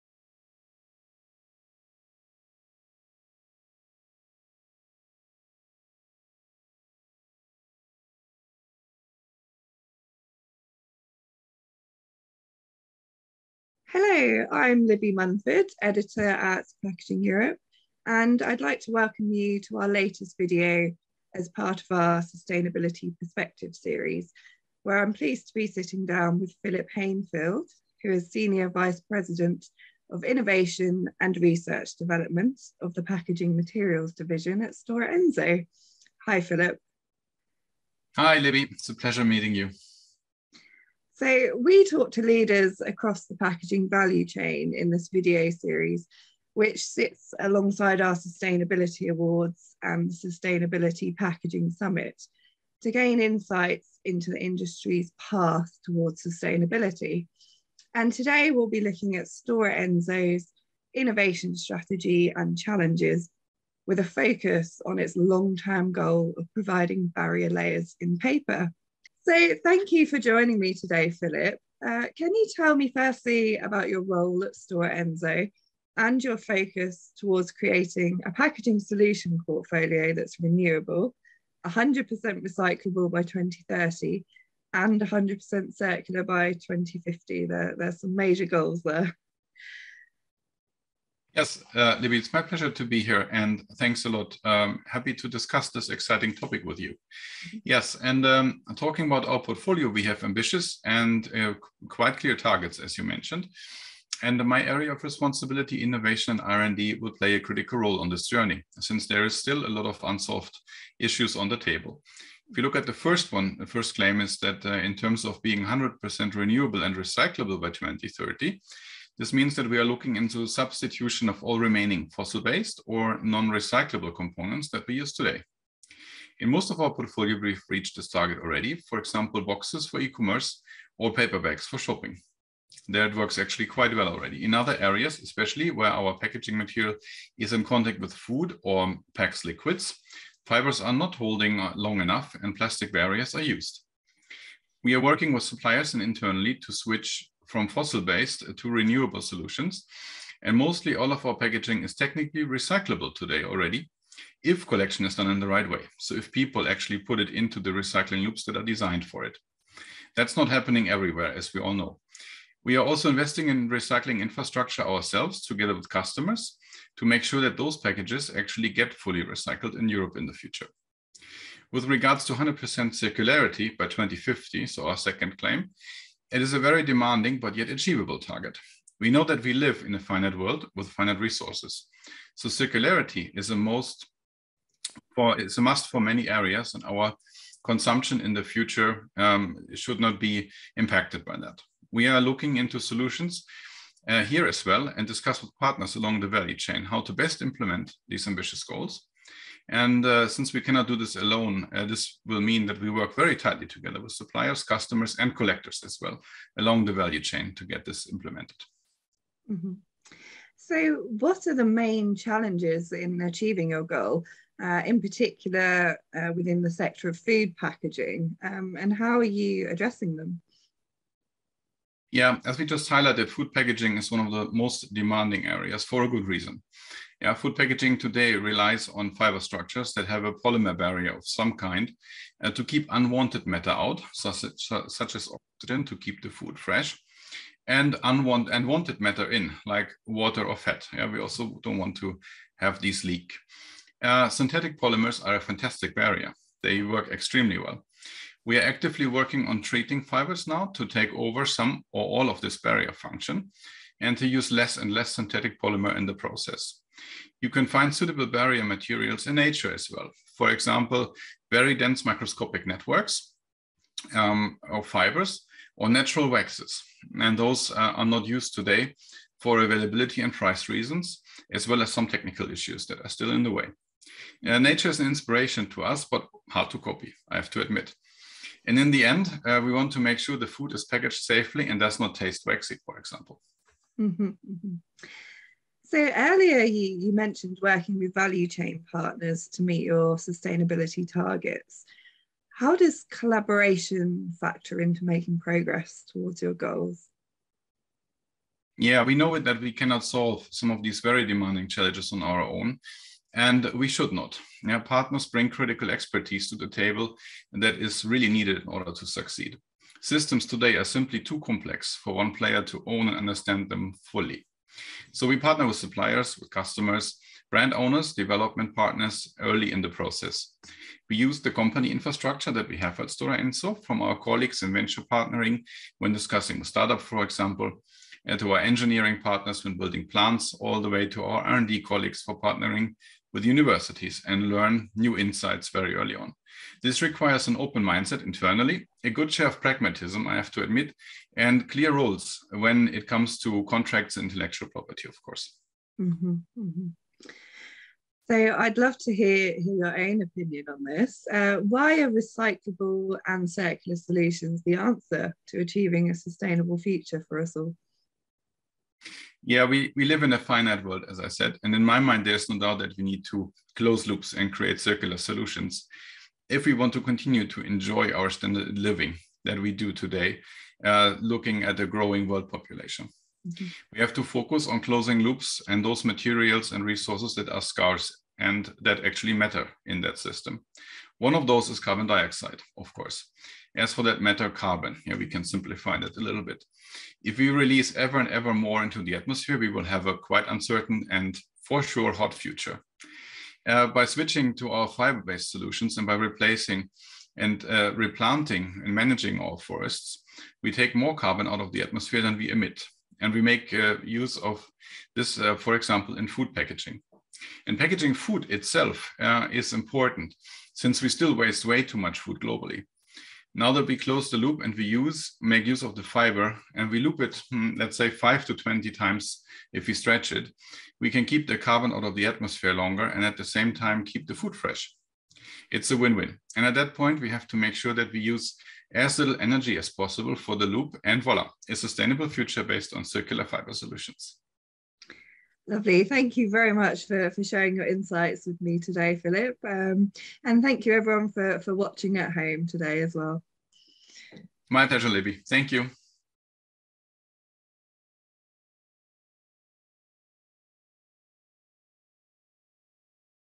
Sustainability Perspectives with Stora Enso
Phillip Hanefeld – Senior Vice President of Innovation and Research & Development, of the Packaging Materials division at Stora Enso, discusses strategies and challenges in our latest video of the Sustainability Perspectives series.